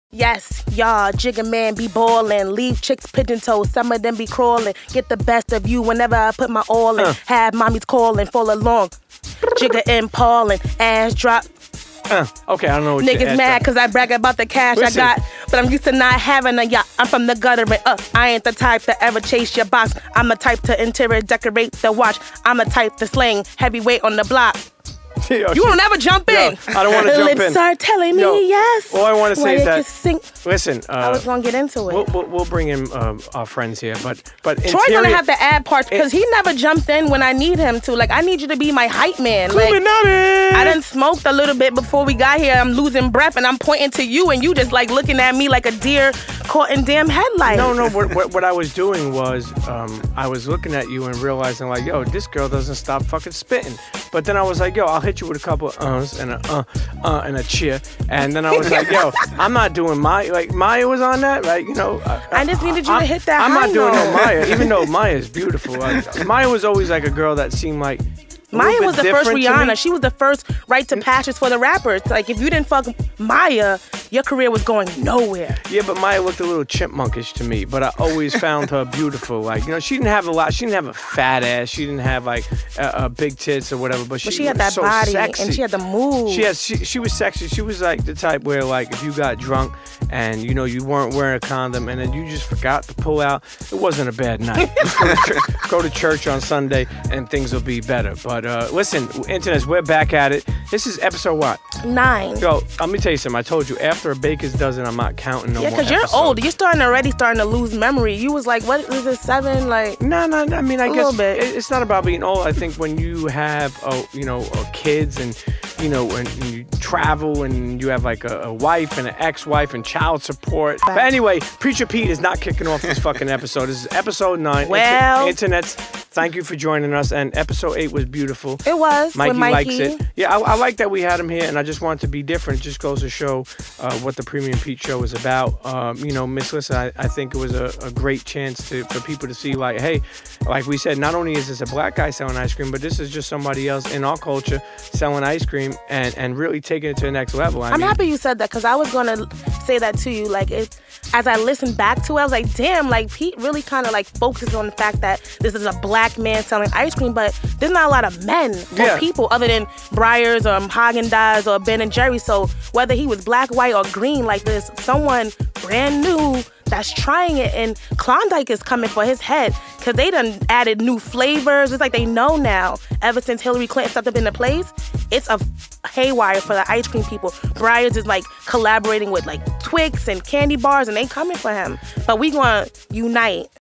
RAPPERS RITE OF PASSAGE - The Premium Pete Show (feat. Miss Lissa Knows)
EXCLUSIVE footage of Miss Lissa Knows Spitting her weekly Bars. Episode 9 "Kosher Talk" we're joined by ItsTheReal and in this clip we kick off episode 9 getting into some bars, ice cream, and Mya (no pause needed) Check us out on iTunes, SoundCloud, Sticher, Tunein, and Google-Play. Tell a friend to tell a friend, CHEA.